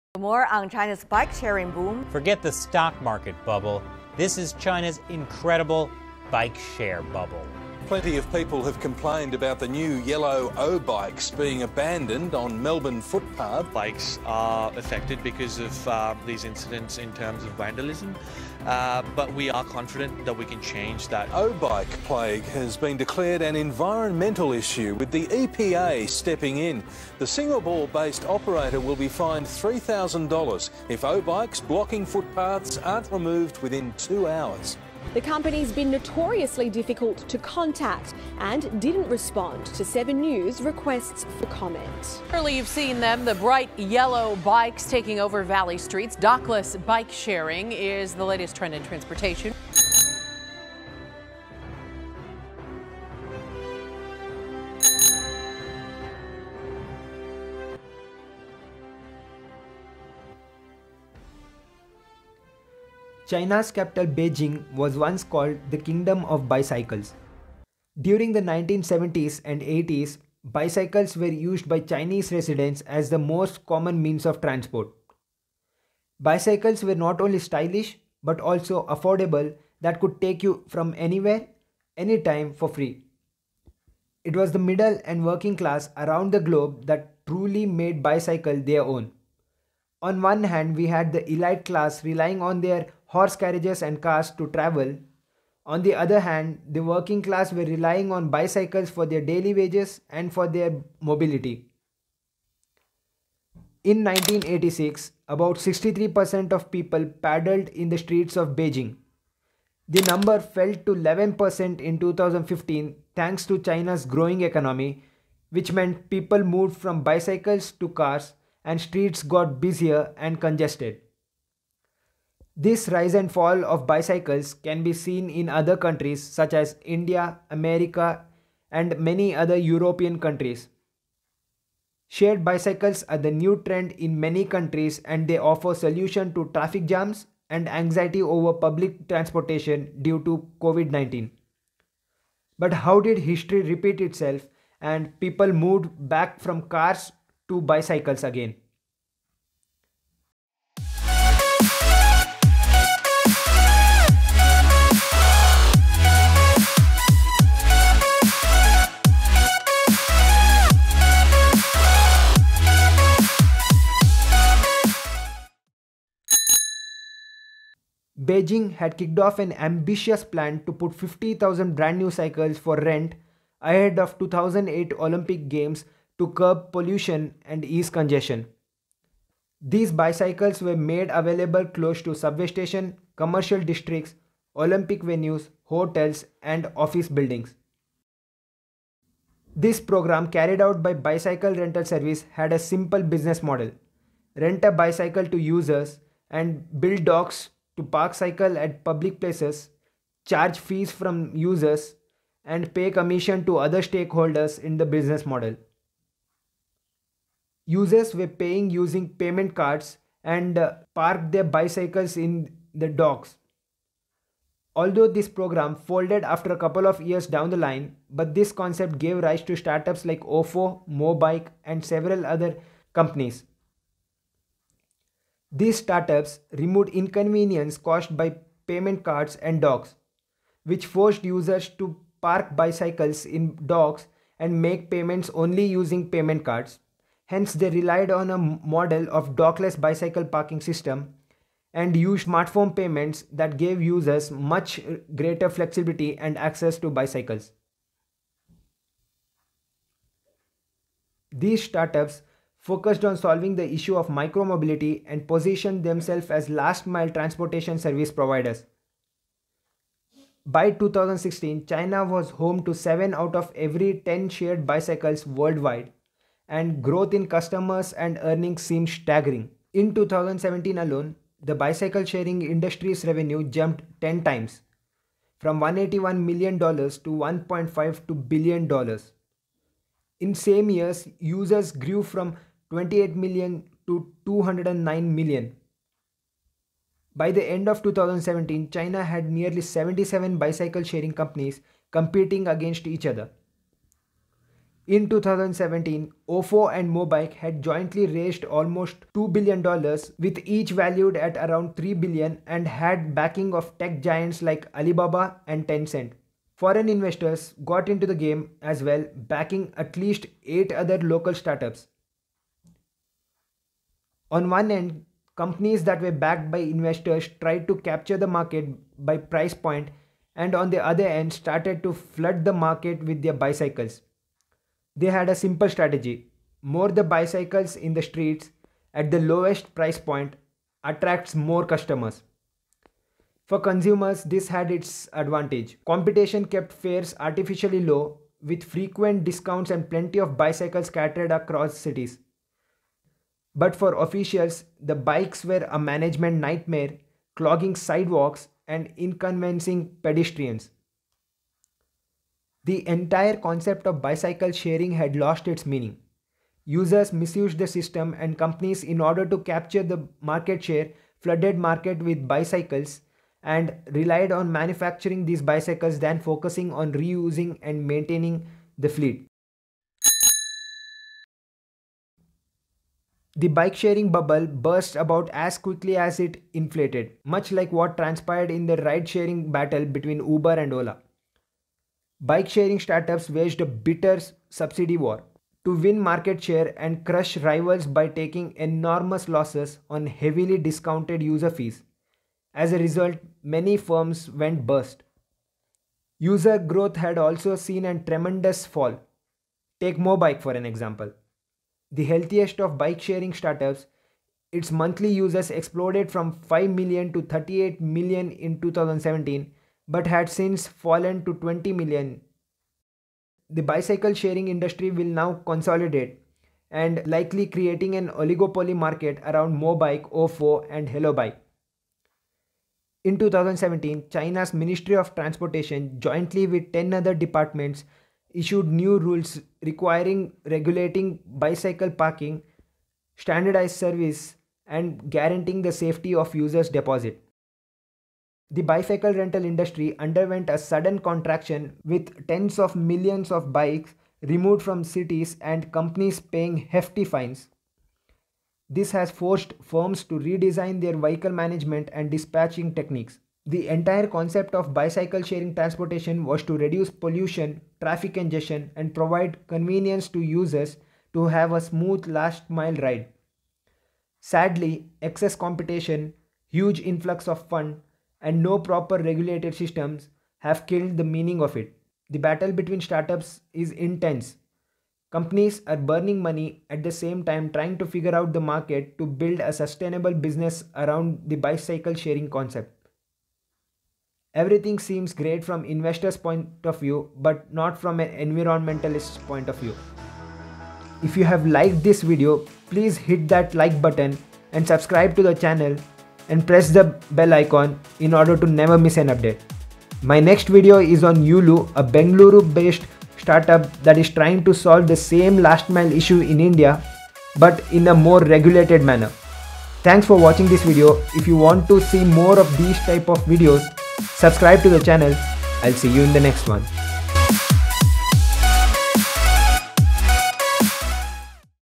From trend to disaster | Detailed case study on bicycle sharing services
In this video I have tried to explain how the humble mode of transportation which was used to reduce pollution has turned into environmental crisis due to the new trend of bicycle sharing concept. This concept was pioneered in 2008 in China but turned out to be a bubble and made a huge impact on planet earth. Companies like OFO, Mobike, Hello bike etc. are few of the startups competing in this market.

🕒 Timestamps
0:00  Intro
0:52  What is bicycle sharing trend
2:47  Rise of the bicycle empire
6:06  Fall of the bicycle empire
7:21  Bicycle an environmental issue